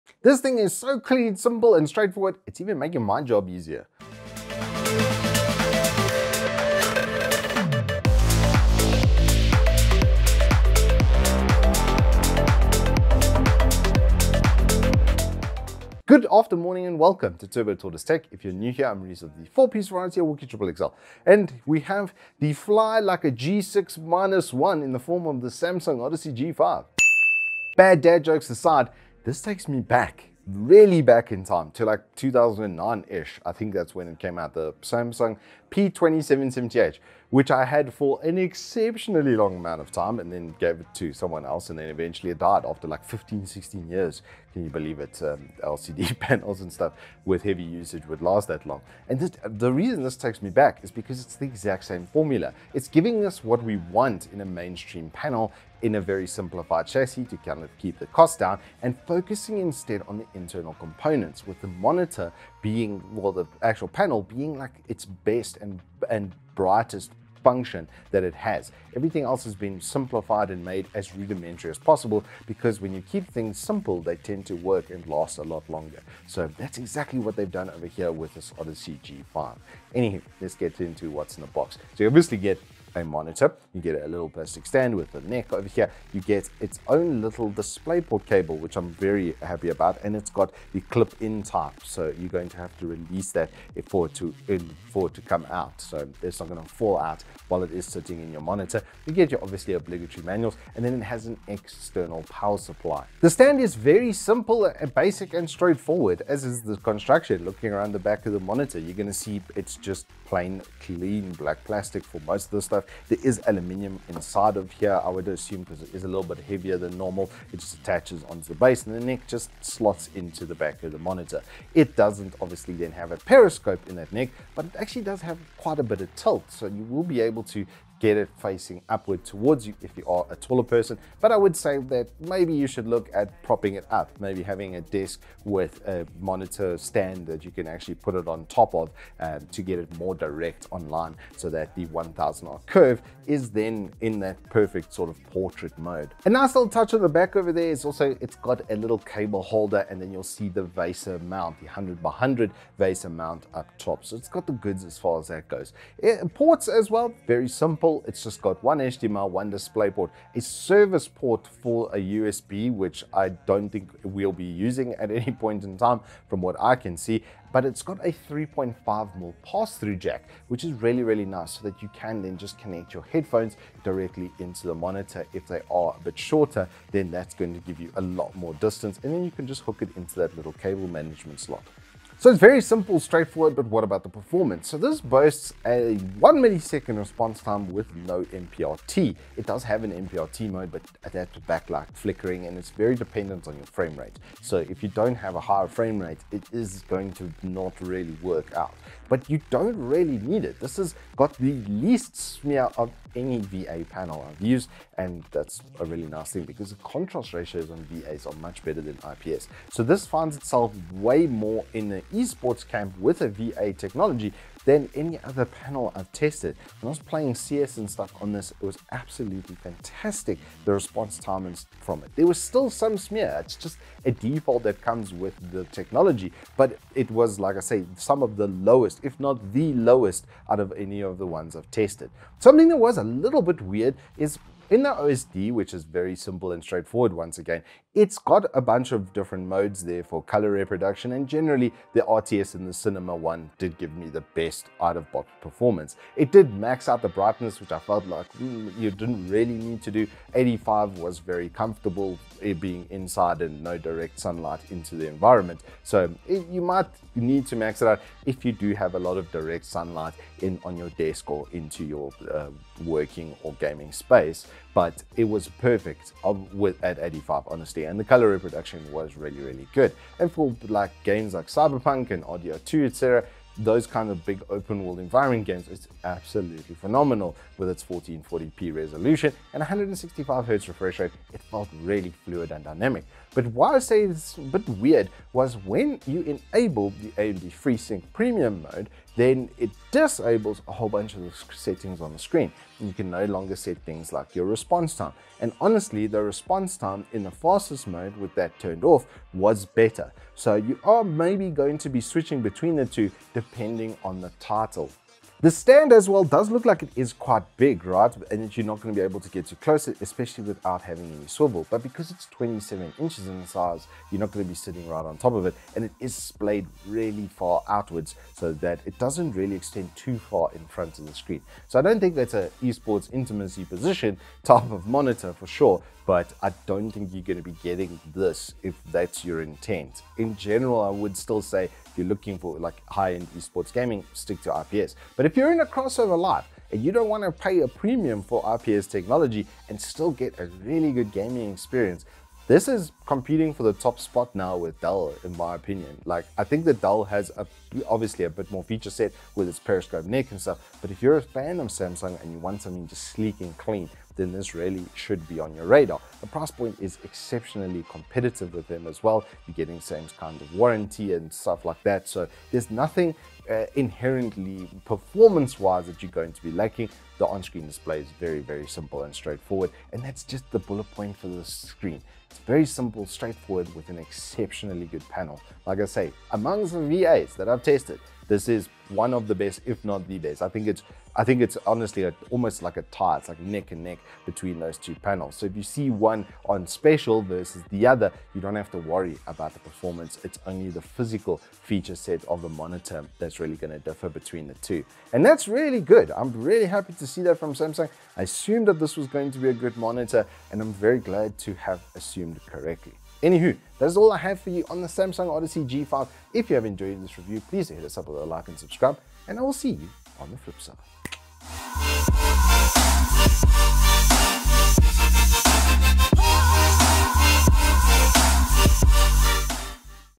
An absolute CLASSIC VA based setup from Samsung - Odyssey G55A Review
Gooooooooood after morning and welcome to another Turbo Tortoise Tech review!

VA technology has become the default for mainstream panels since the death of TN. IPS has become that premium setup and the VA the best for budget buys. This is now 1 of the ways to do it with the 1000R curve 27" Samsung Odyssey G5. The frame is very basic and straight forward with the budget being spent on the panel which is 1 of the best VA's the Wooks has ever tested. Join our main mad lad for the full review right here on Evetech!

Agree or disagree? We would love to hear your thoughts on it in the comments down below!

For more awesome gaming and tech content as well as spicy giveaways follow us on the socials below:

0:00 Intro
0:29 First Impressions
1:20 Package Contents
1:43 Physical Features
2:36 Ports
3:03 Performance and Usage
3:55 OSD and Functions
5:06 Usage Conclusions
6:43 Outro